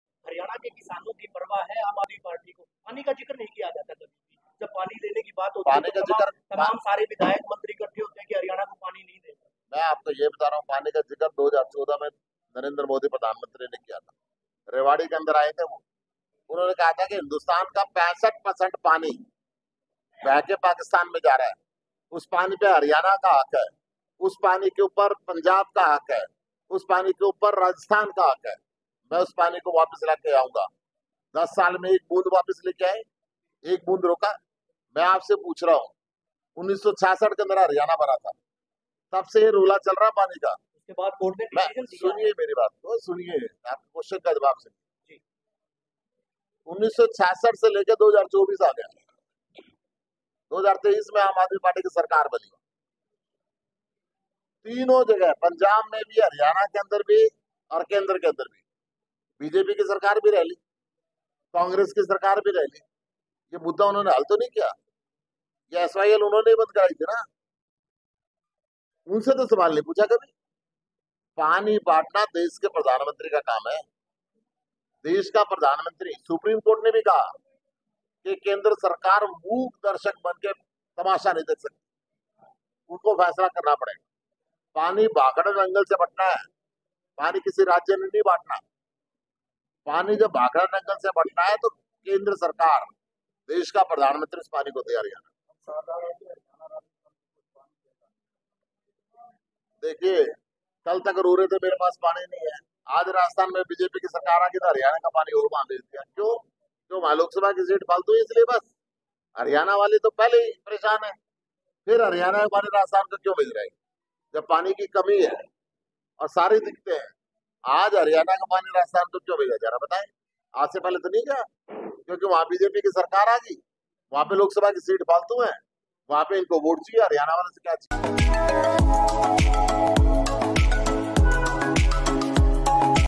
SYL मुद्दे को PM Modi सुलझा सकते हैं, लेकिन वह मौन बनकर बैठकर हैं- Sushil Gupta | India News Haryana
आम आदमी पार्टी के प्रदेश अध्यक्ष सुशील गुप्ता आज नारनौल पहुंचे थे. नारनौल पहुंचकर प्रदेश अध्यक्ष सुशील गुप्ता ने SYL के मुद्दे को लेकर कहा कि भाजपा की केंद्र सरकार इस मुद्दे पर मौन बनकर बैठी है. प्रधानमंत्री चाहे तो इस मुद्दे को सुलझा सकते हैं, लेकिन केंद्र सरकार कभी भी इस मुद्दे को नहीं सुलझाएगी. भाजपा सरकार ने ही किसानों को पैसा वापस किया था.